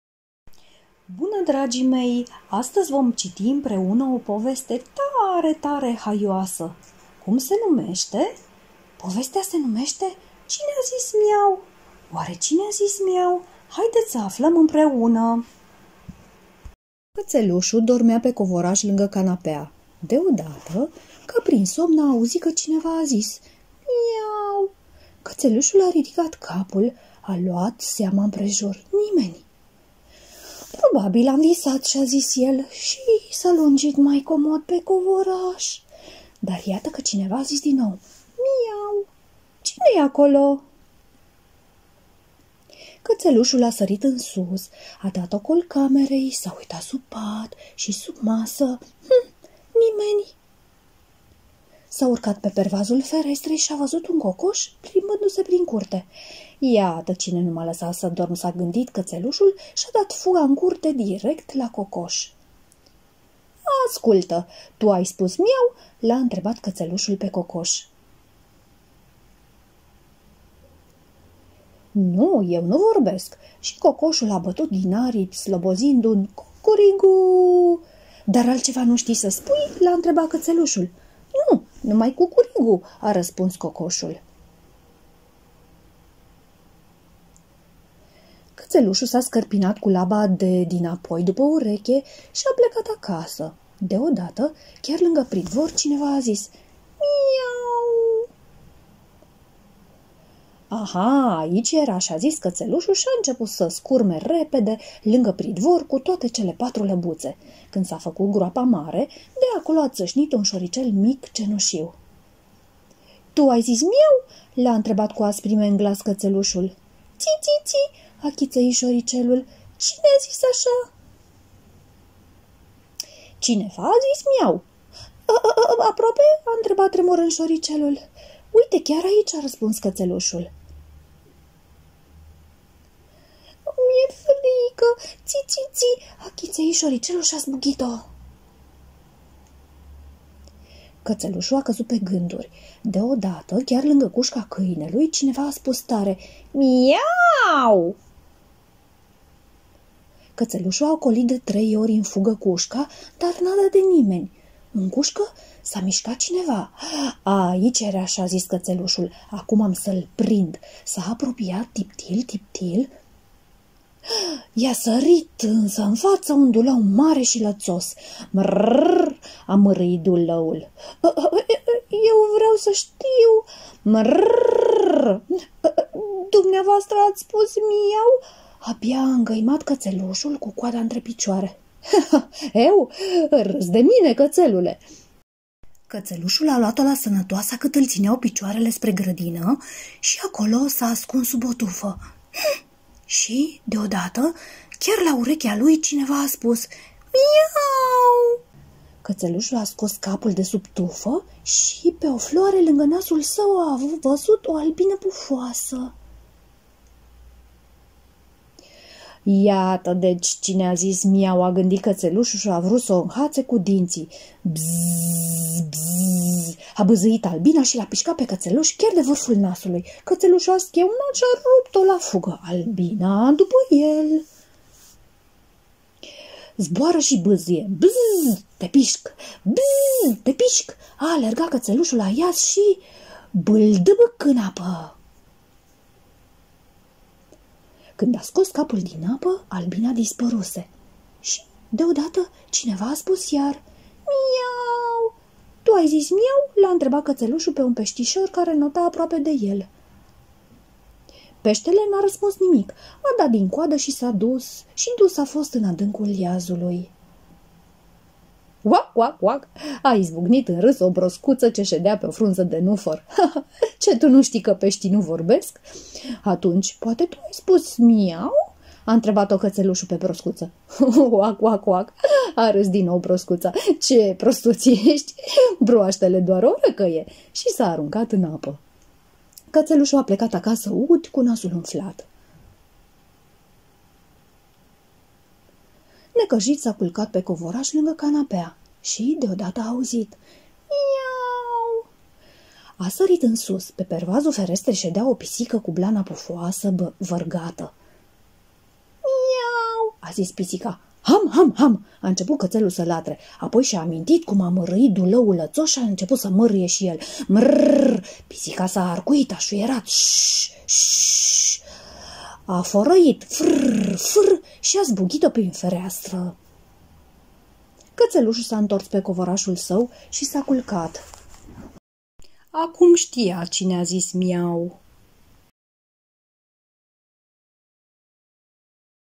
Cine a zis miau? de V. Suteev
O poveste minunata scrisa de Vladimir Suteev, o poveste potrivita pentru copilasii de 1.5-3 ani